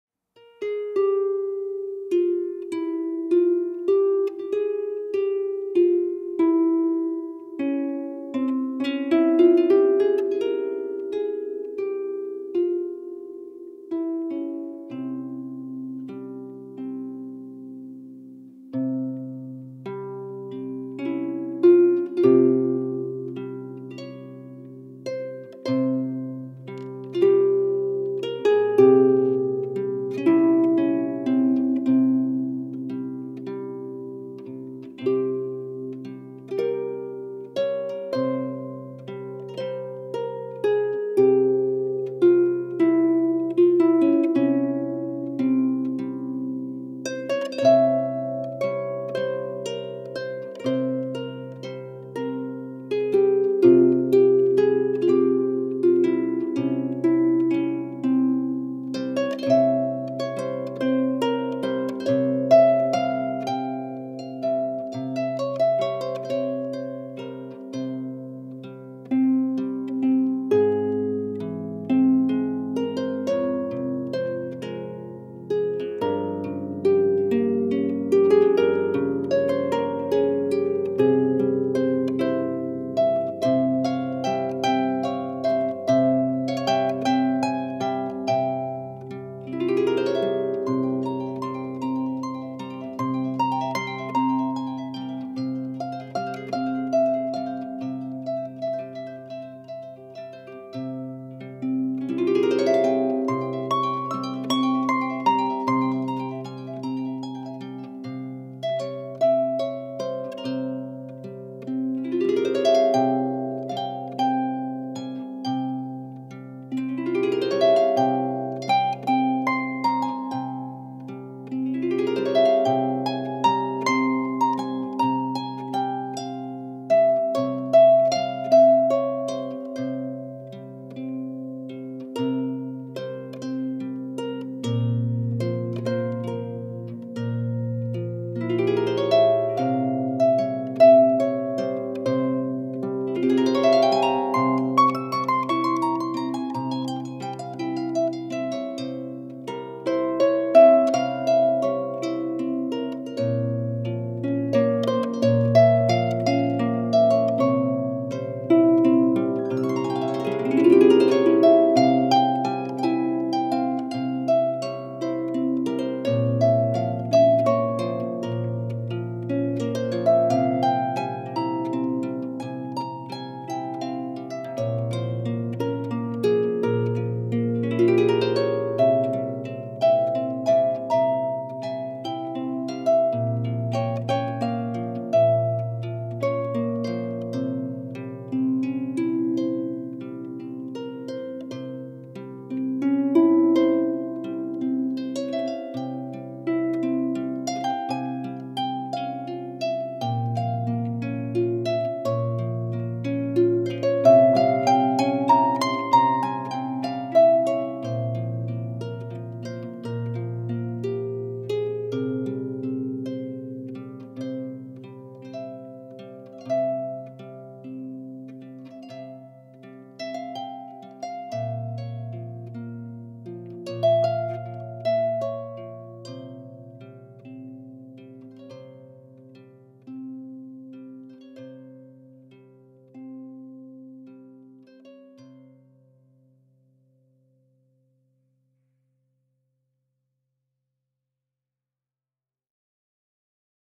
#1 Harp Healing by LIdia Stankulova Middle Eastern Mode
Follow your own needs,
pass through what makes you happy,
say thanks to everyone and everything,
say thanks for yourself
Take care of your soul
Peace & Love
Lidia Harp Healing

Recording :
Home studio, first attempt.
Not successful, didn't upload :D
Used few photos of my fiend Nishani, LaFairyByNish